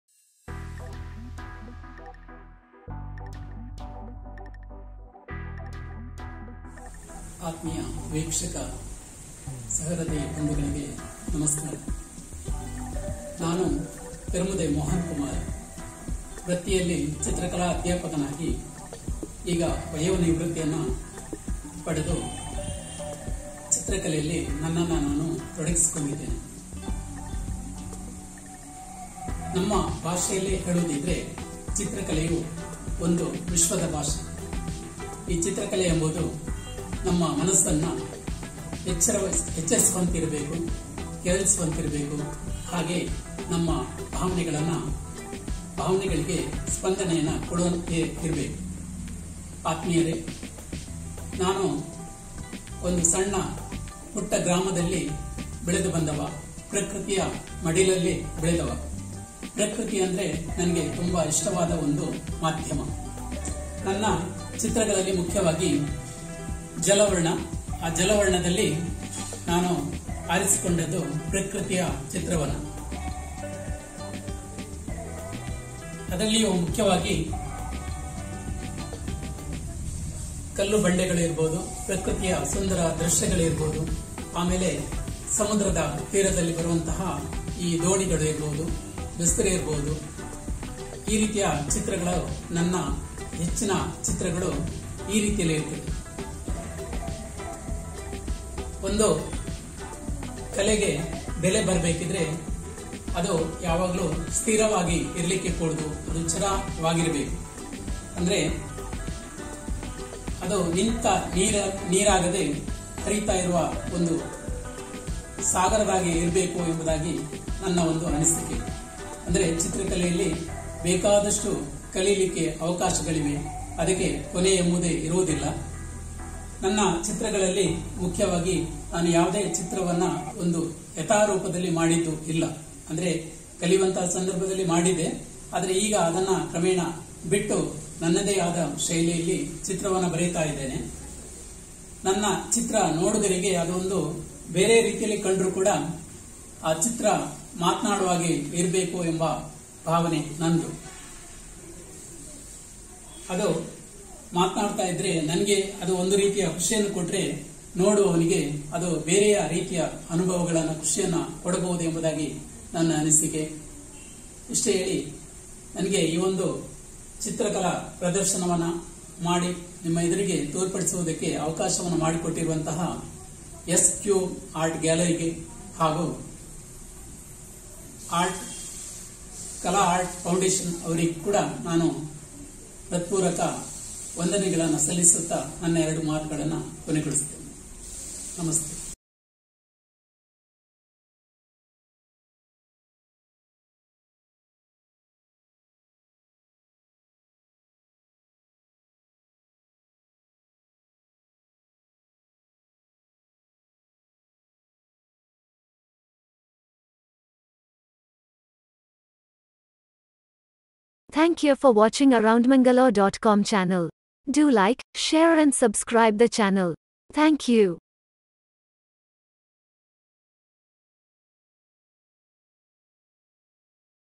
Art of the Week (2) – Boat Series by Permude Mohan Kumar – 9th to 14th Aug 2021 – S Cube Art Gallery
Boat Series by Permude Mohan Kumar

Art of the Week (Series 2)

9th to 14th Aug 2021

S Cube Art Gallery at Kodialguthu Center for Art & Culture
G. G. Road,
Ballalbagh,
Mangalore